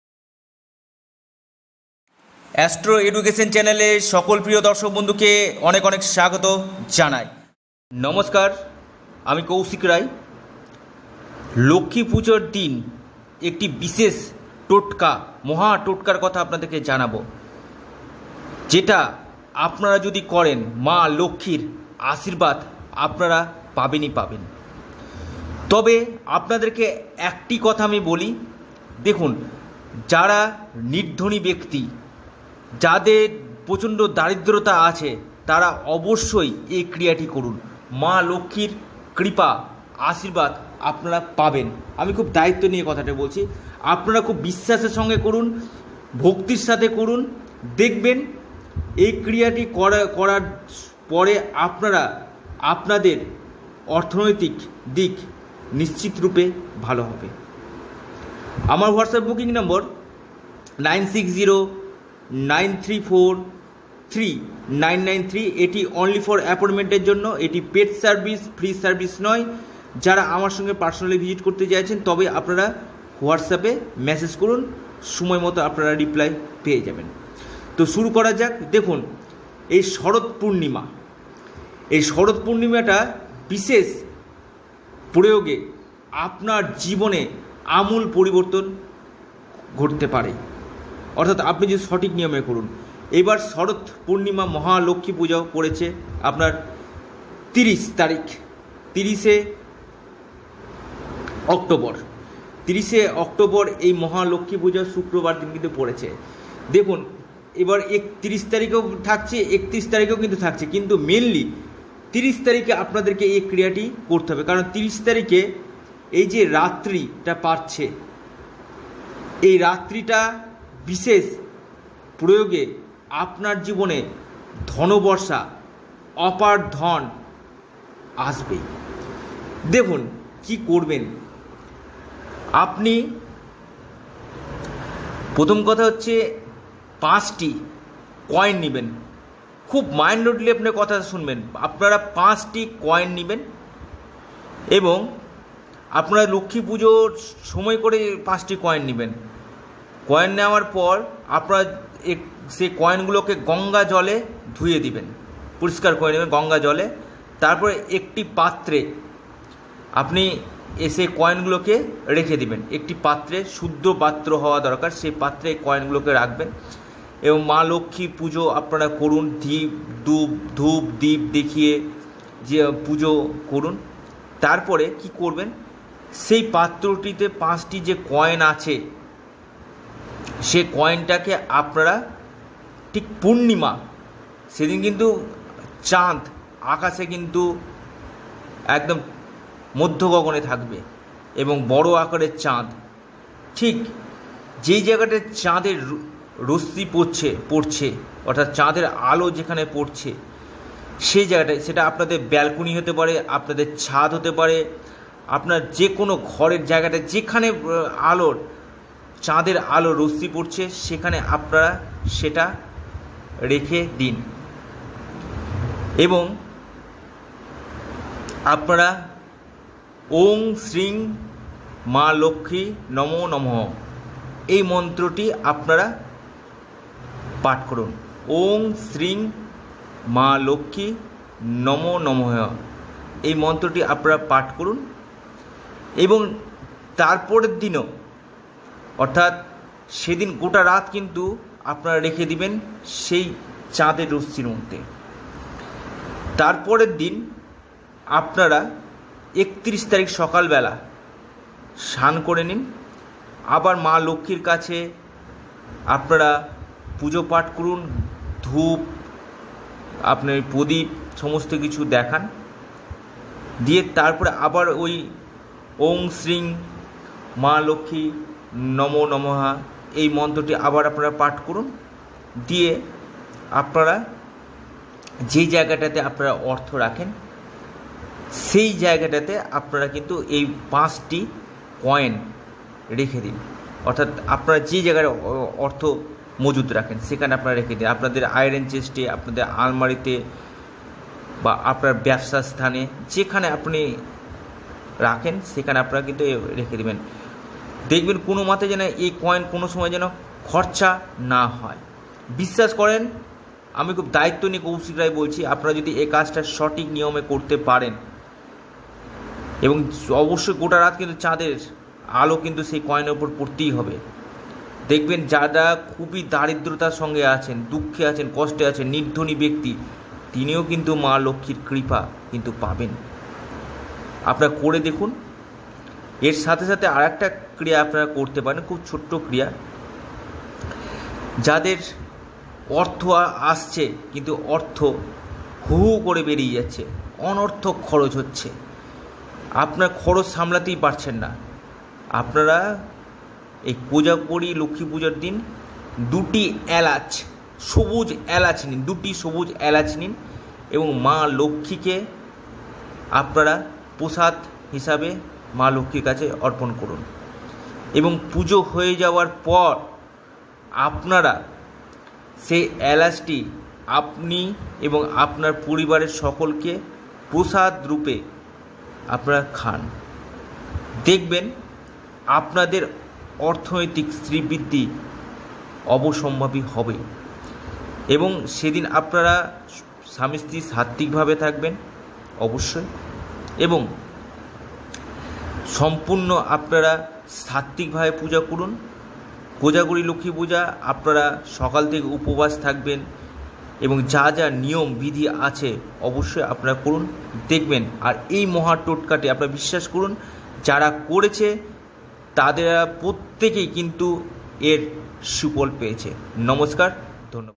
শরৎ পূর্ণিমা মহা প্রয়োগ! মা লক্ষ্মীর আশীর্বাদ!!
লক্ষ্মী পূজার দিন এই মহাঁপুর একটি করুণ ধনসম্পত্তি বৃদ্ধি পাবে!